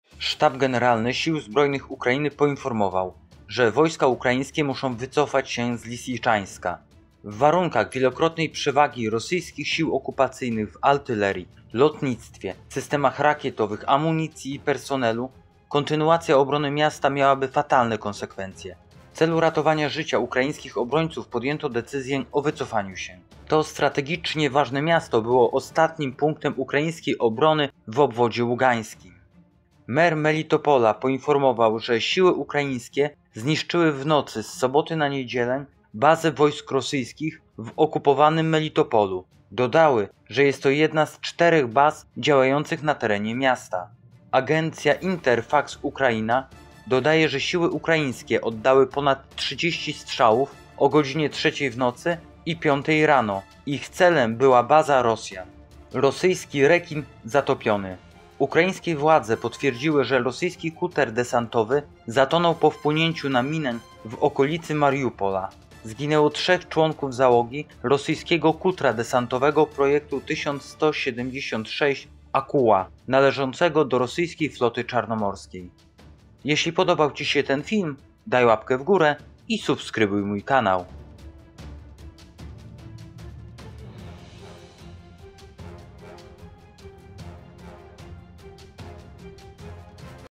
UKRAINA - PILNE! - Lisiczańsk Upadł - Zniszczona Baza W Melitopolu - Rosyjski Rekin Zatopiony
Wojska Ukraińskie wycofały się z Lisiczańska.
Siły ukraińskie zniszczyły bazę wojsk rosyjskich w okupowanym Melitopolu.
Rosyjski "Rekin" zatopiony.